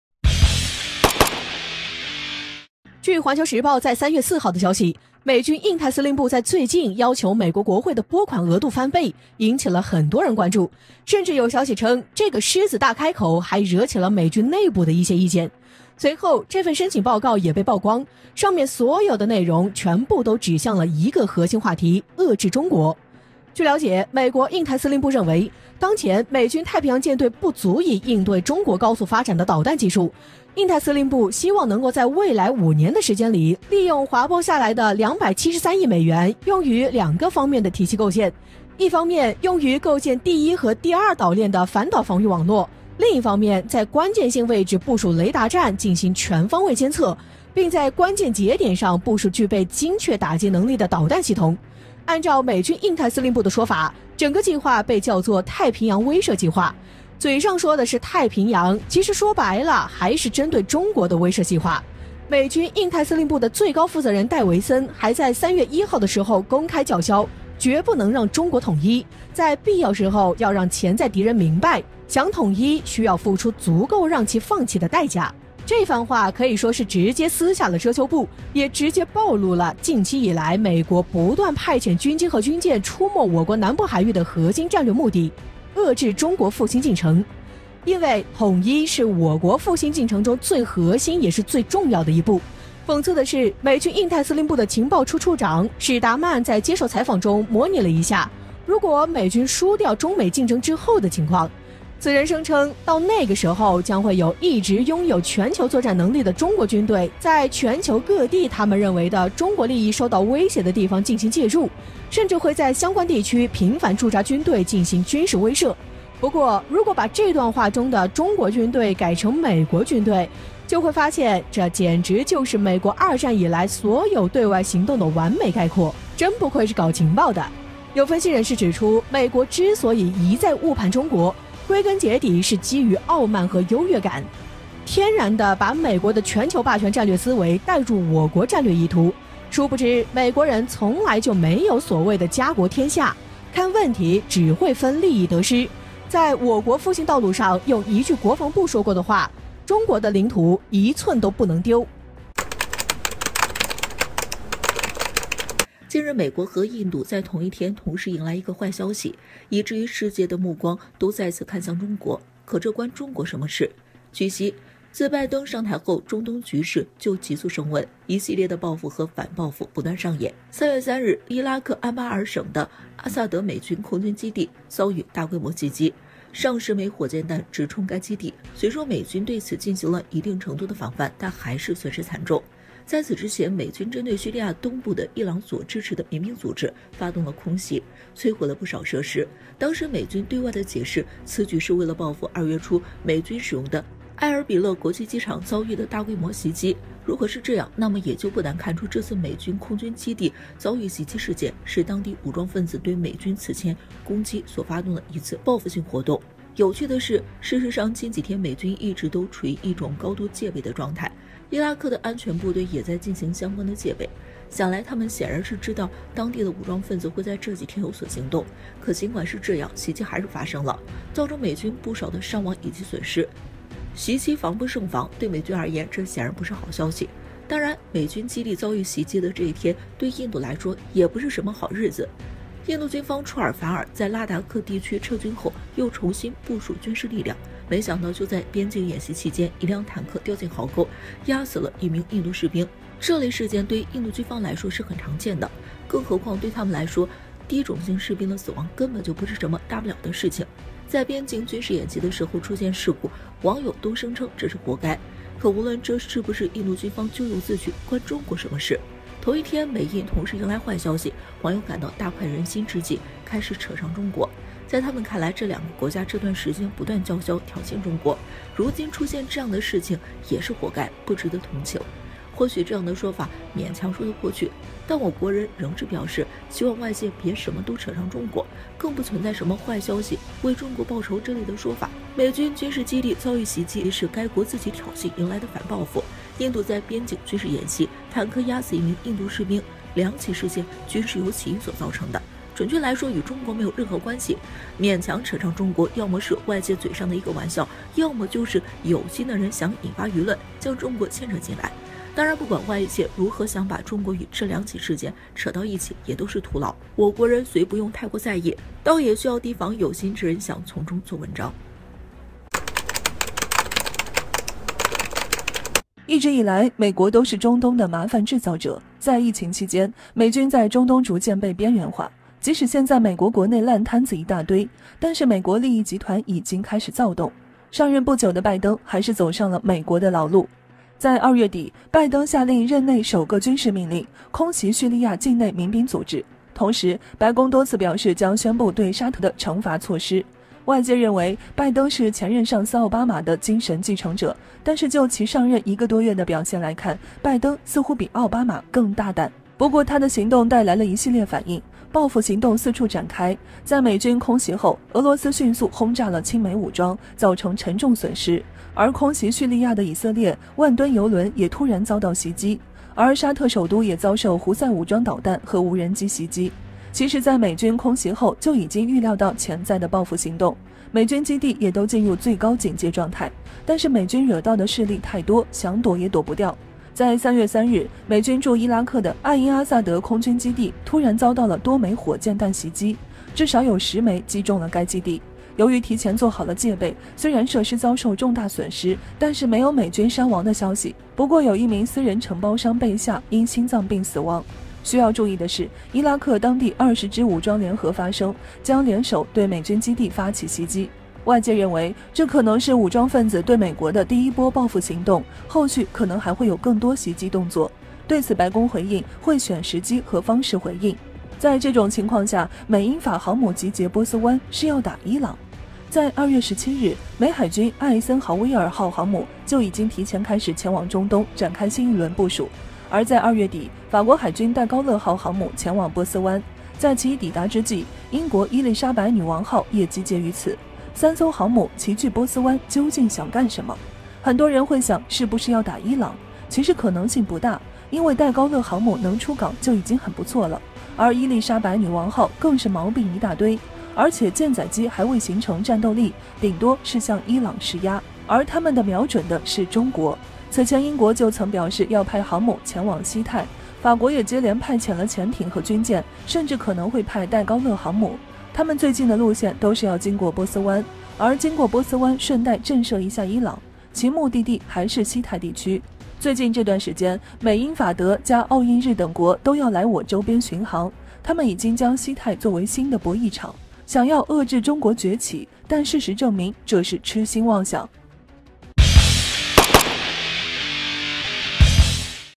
同一天，美印同時迎來壞消息，可這關中國什麼事？網友：活該！【一号哨所】
1、00:00-02:30 美國徹底撕下遮羞布，印太司令部公開目的：不能讓中國統一
2、02:31-05:12 美印同時迎來一壞消息，是報應？國人：別什麼事都扯上中國！
3、05:13-08:02 美英法航母集結波斯灣，是要打伊朗？其實瞄準的是中國
同一天，美印同時迎來壞消息，可這關中國什麼事？網友：活該！